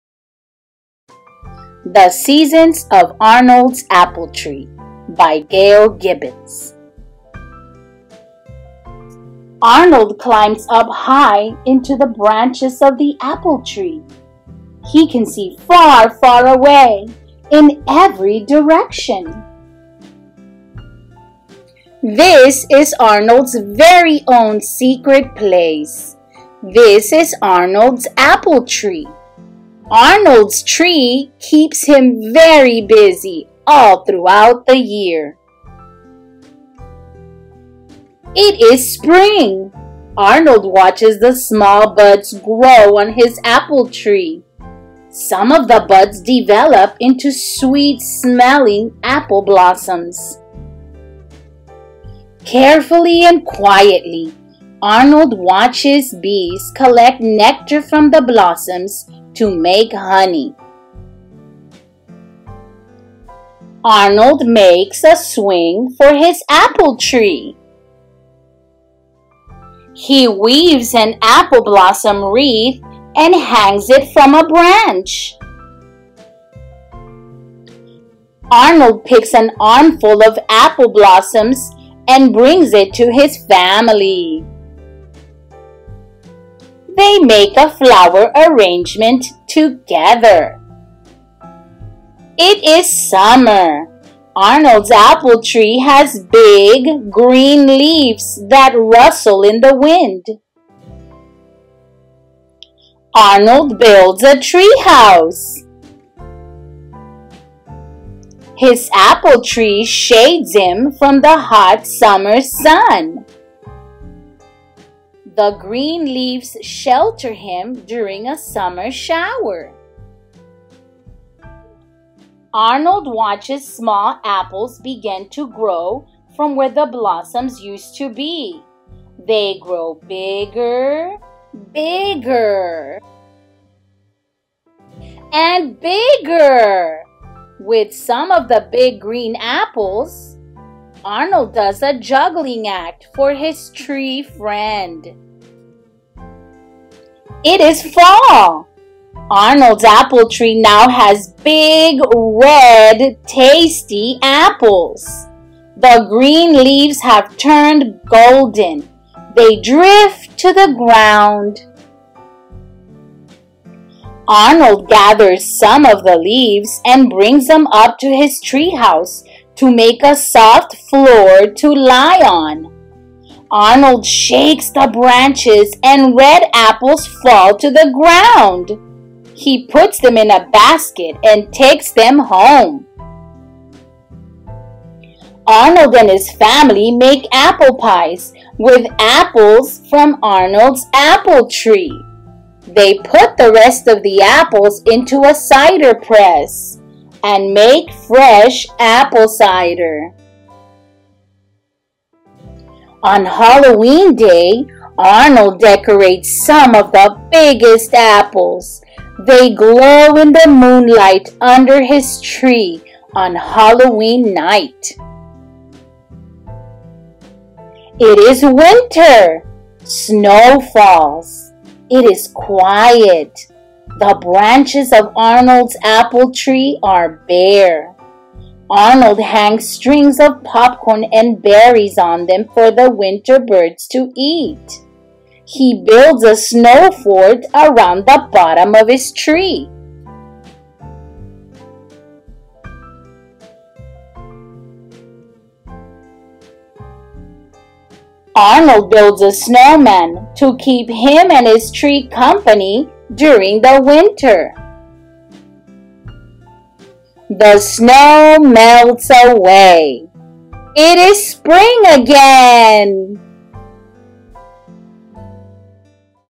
The Seasons of Arnold's Apple Tree
by Gail Gibbons
Read by Maria E. Hinojosa